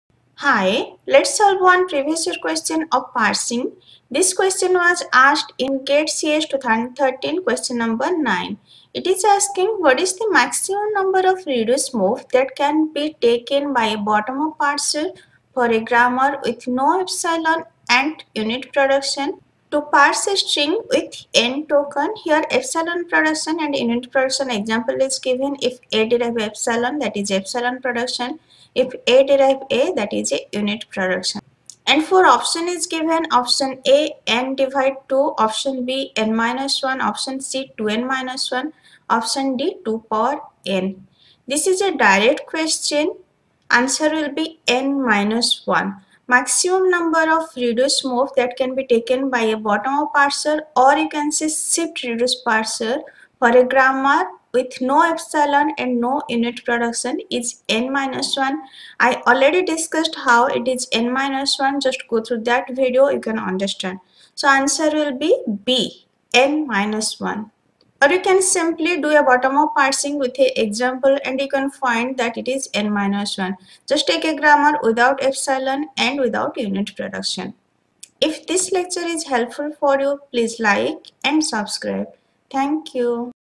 GATE CS 2013,Q9: What is the maximum number of reduce moves that can be taken by a bottom-up parser
GATE CS 2013,Q9: What is the maximum number of reduce moves that can be taken by a bottom-up parser for a grammar with no epsilon- and unit-production (i.e., of type A → є and A → a) to parse a string with n tokens?